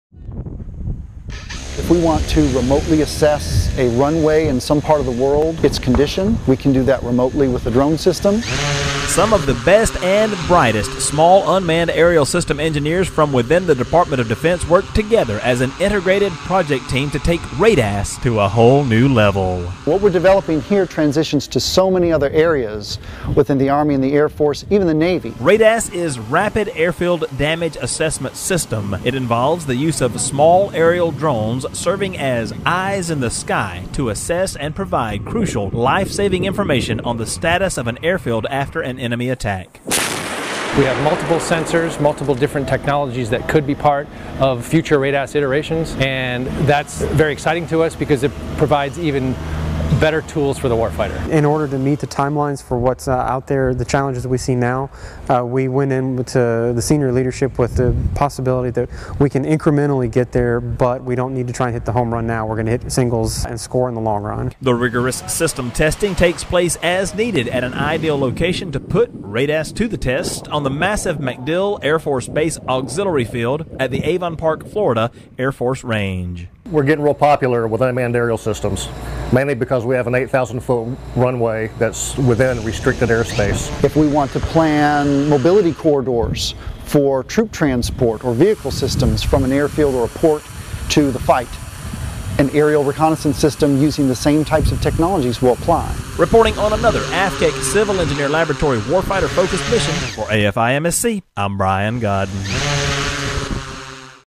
Advanced RADAS testing takes this Warfighter safety-focused drone technology to the next level
The Air Force Civil Engineer Center's Requirements, Research and Development, and Acquisition division at Tyndall Air Force Base is part of a special team taking RADAS to the next level to ensure Warfighter Readiness and safety. Air Force Installation and Mission Support Center public affairs specialist, Brian Goddin gives us a birds-eye view of this advanced technology!